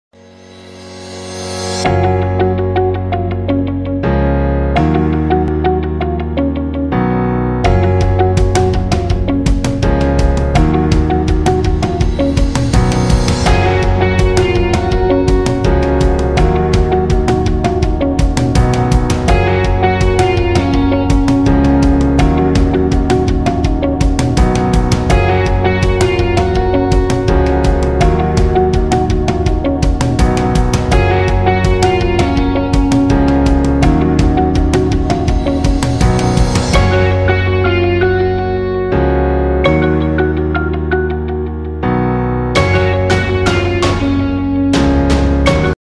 FTC team has completed the installation and operation of sliding gate motors
Lock your home with FTC ...
FTC
keeps Going and Going...
address: صور- جل البحر- جنب مختبارات المشرق.